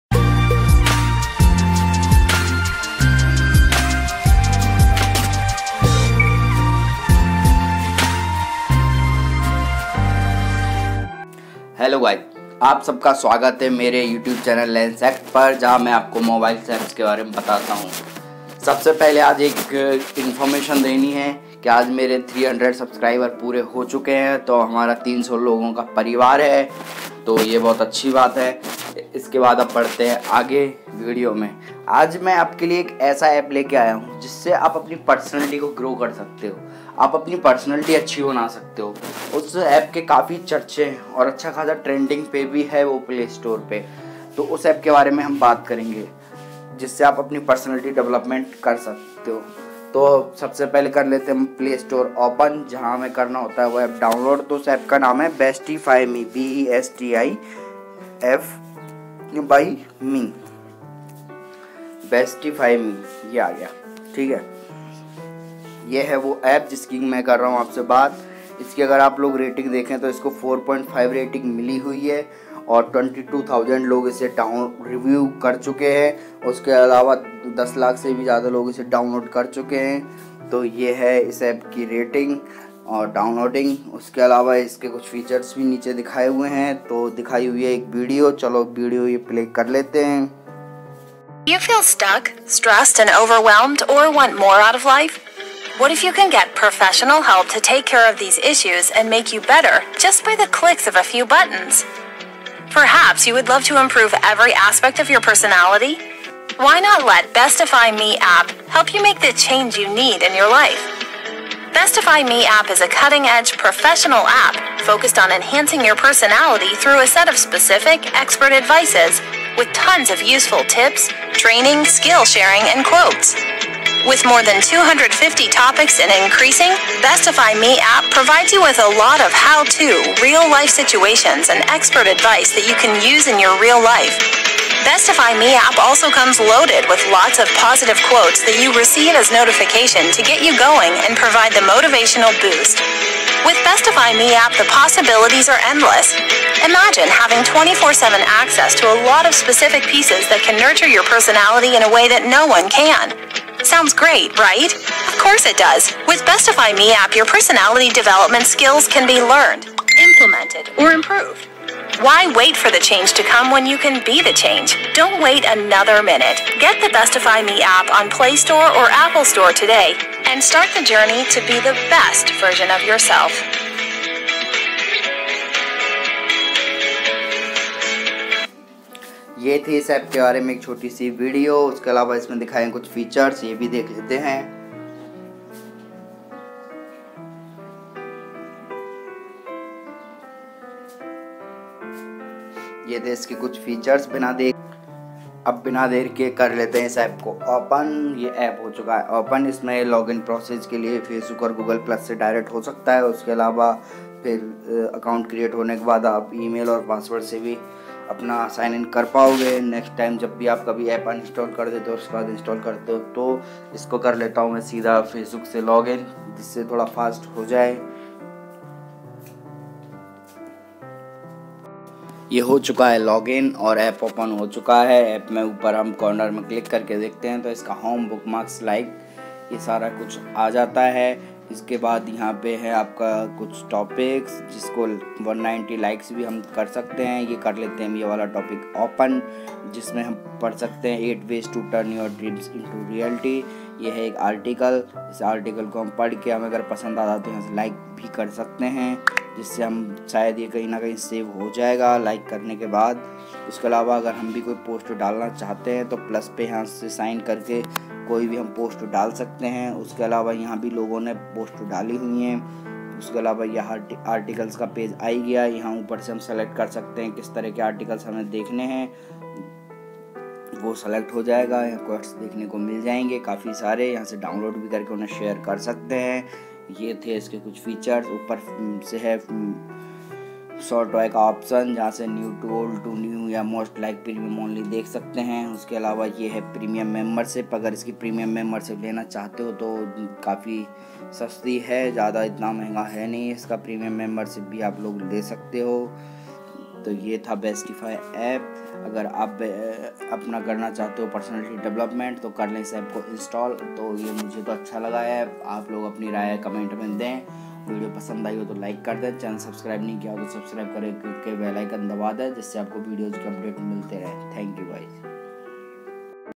Best app for Personality development | BestiFy Me App | Lans Act
Personality Development is a professional app focused on enhancing your personality through set of specific expert advice with tons of useful tips, training, skill sharing and quotes. Based on real-life survey, the app truly focuses on how to improve every aspect of your personality. Just read carefully, follow every advice and start a process of self-development and improve your personality significantly.

★ PERSONALITY DEVELOPMENT TRAINING ★

The app can be your personality development training guide. There are a lot of how-to, real-life situations and expert advice that you can use in your real life. The development of a good personality that will win in every aspect of your life is not easy, but the app is here to be your specific guide! There are more than 180 topics included and each content you read can be shared directly via email or social media.

★ LEARN, IMPLEMENT AND IMPROVE ★

The app is not specifically a book, but it can be rated as one of the best books for personality development. There are a lot of specific pieces that can nurture your personality in a way, no one can! Your personality development skills can be learned, implemented or improved, and that is what this app tends to do!

★ INSPIRATIONAL QUOTES ★

We know, text, tips and photos sometimes are not enough in this complicated and dynamic world. Thus, we’ve included inspirational quotes as well. Expects some of the best quotes to be found here and you can also receive them as a notification. We care about your personality development, so there will be a lot of positive quotes to get you going and motivate you!

★ PERSONALITY DEVELOPMENT FEATURES: ★

✔️ Professional guide and training for personal development skills
✔️ Easy to understand content for various personal development tools
✔️ Great full professional training to improve personality
✔️ Inspirational quotes included
✔️ Any content can be shared
✔️ Any quotes can be saved in mobile.
✔️ Like articles
✔️ Bookmark articles to read them later
✔️ Request topics
✔️ Get suggestions
✔️ Share your thoughts with 1M+ users
✔️ FREE to use Personality development app

Don’t wait for the change to come, be the change.
Start your personality development now.
Get the app for FREE